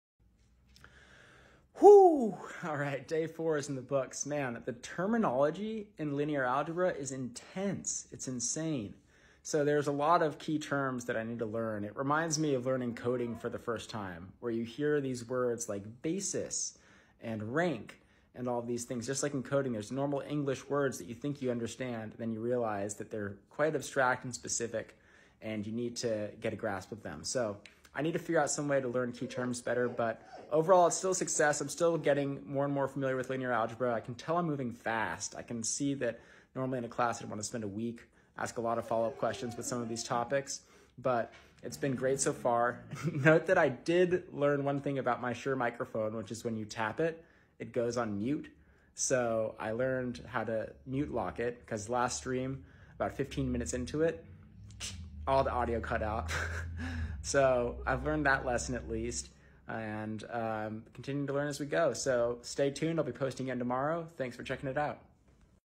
Just when I thought I knew... The mysteries of Linear Algebra!
Basis, rank, span... the list goes on... reminds me of the trickiness around learning 'model', 'hook' and other words learning to code.. Well I'll keep trucking on!